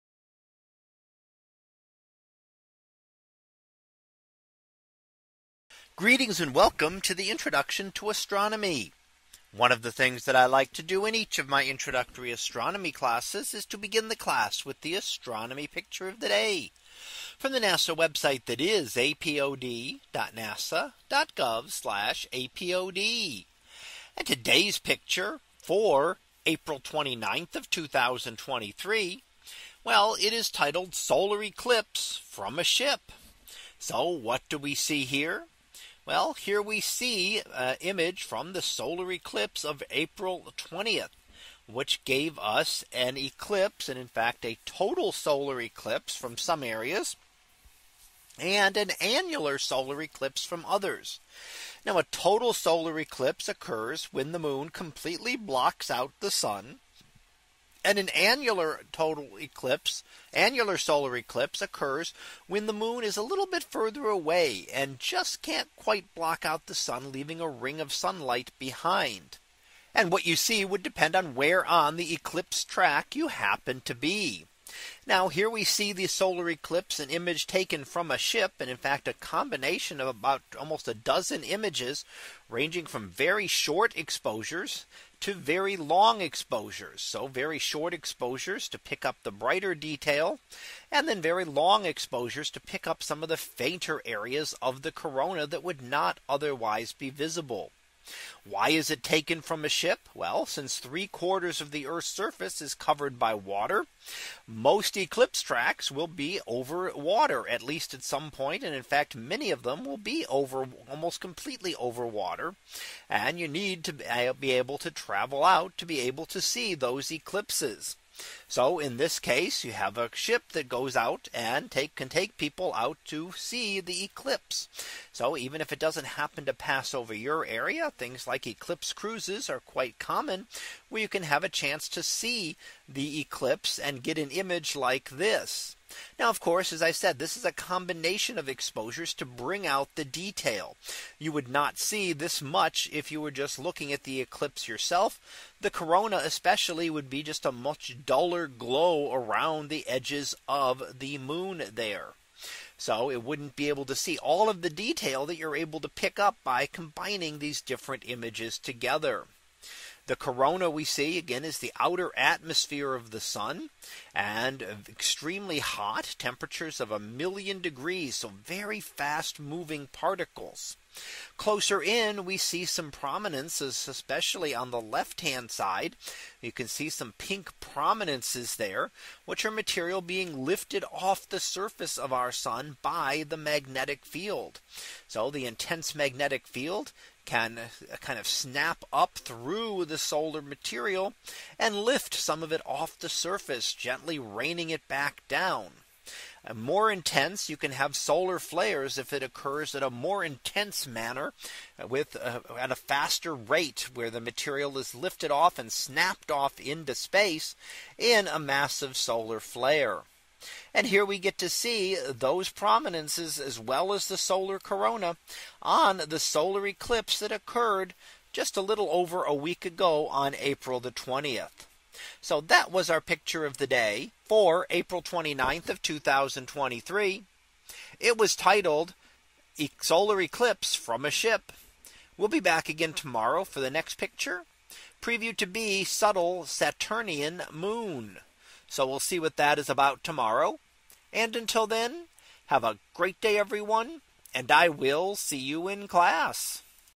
2023 April 29 - Solar Eclipse from a Ship
In today's image, we see an image of the solar eclipse from April 20 of this year. In a solar eclipse, our Moon passes in front of the Sun temporarily blocking out its light. This image was taken from a ship off the west coast of Australia and consists of nearly a dozen images of various exposures to bring out the detail in the solar corona.